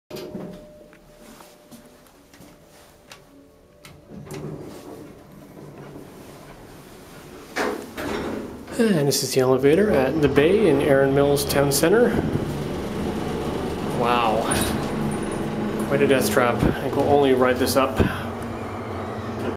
Otis hydraulic elevator - The Bay Erin Mills Town Centre *D E A T H T R A P !!*
Worst elevator I've filmed yet. Shaking, creaking and even slipping as it went up. Must be low on oil or something, but this was ridiculous. Located in The Bay at Erin Mills Town Centre, Mississauga, Ontario. Opened in 1989.